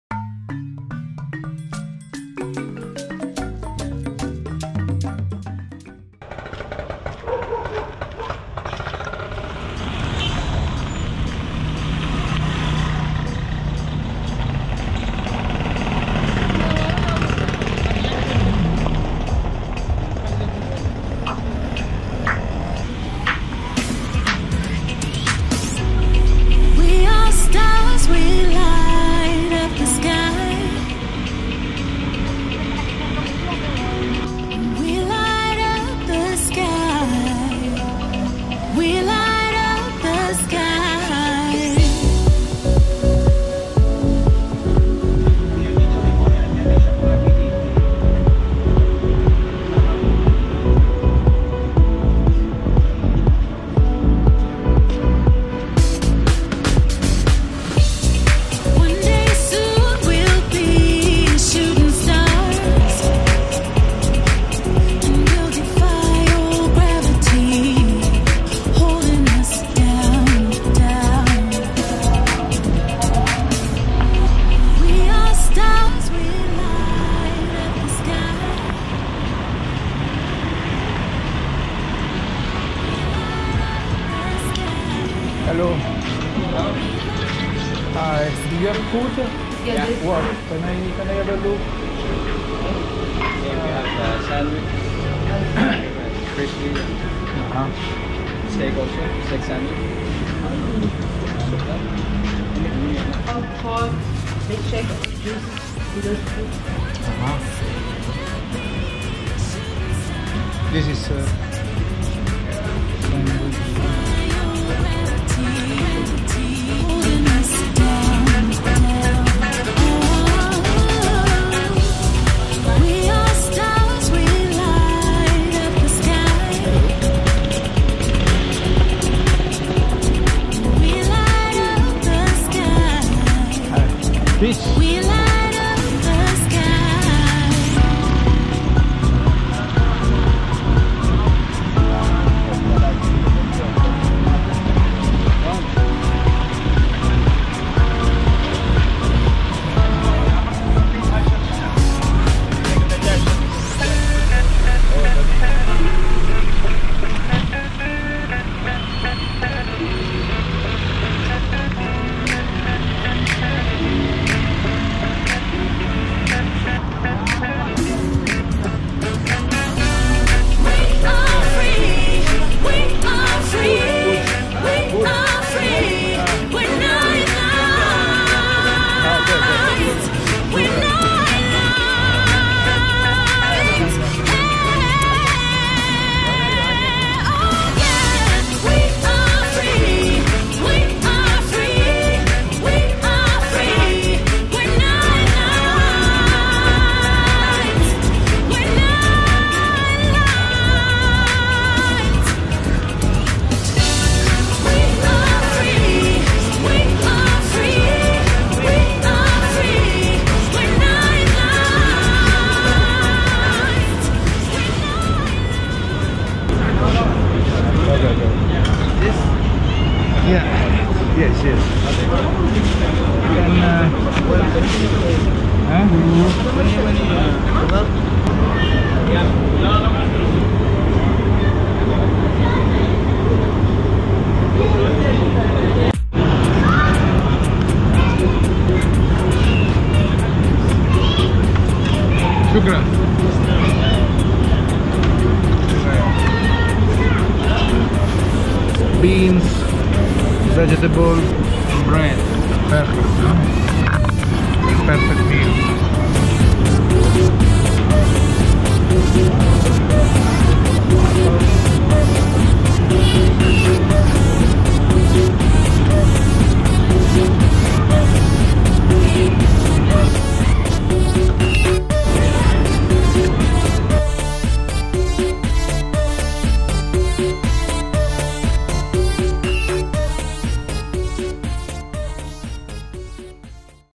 E24 : SUDAN, SUNDAY DINNER IN KHARTOUM
Sudan Capital City, Khartoum by night, having dinner in a Yemenite Restaurant, walking in the city centre at night.